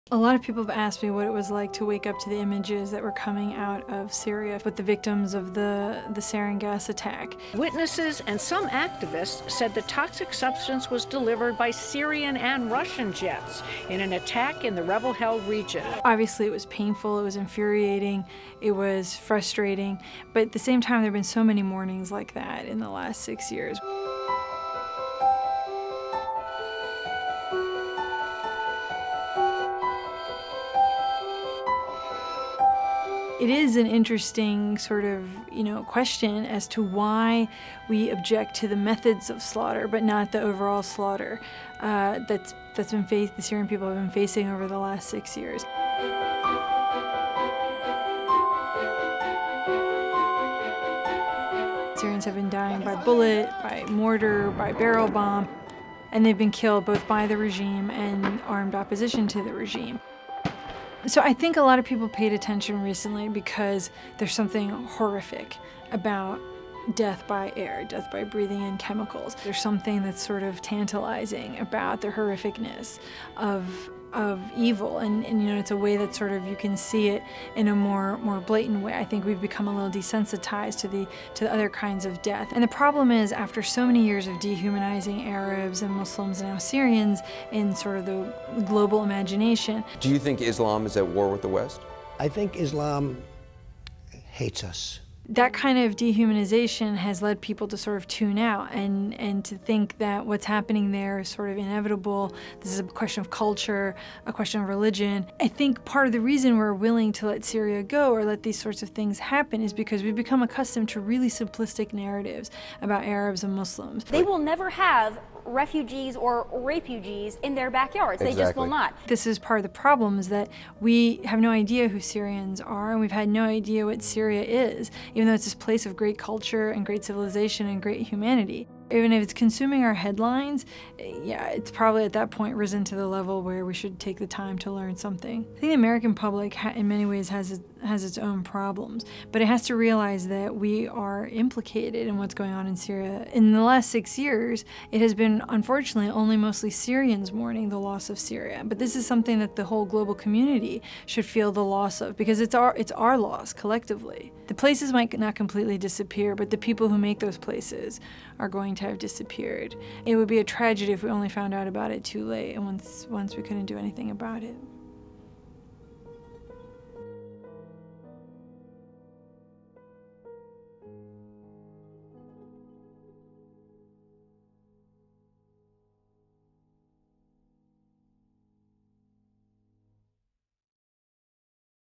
I'm a Syrian-American journalist. Syria is more than the headlines.
Chemical weapon attacks aren’t the only reason to pay attention to Syria.

When Syrian-American author and civil rights lawyer Alia Malek woke up to the images of Syrians dying from a chemical weapons attack in the town of Khan Sheikhoun last week, her immediate response was pain, anger, and frustration.

“But at the same time,” Malek told me, “there have been so many mornings like that in the last six years.”

Since the start of the Syrian civil war in 2011 more than 5 million Syrians have become refugees. Moreover, at least 470,000 Syrians have died, the vast majority by methods other than chemical weapons. Yet it was the chemical attack on Khan Sheikhoun and the Trump administration’s retaliatory decision to launch 59 cruise missiles against a Syrian air base that resulted in heaps of media coverage.

This all led Malek, who has chronicled the story of her family and her time in Syria in her new book, The Home that was Our Country: A Memoir of Syria, to question why “we object to the methods of slaughter, but not the overall slaughter that the Syrian people.“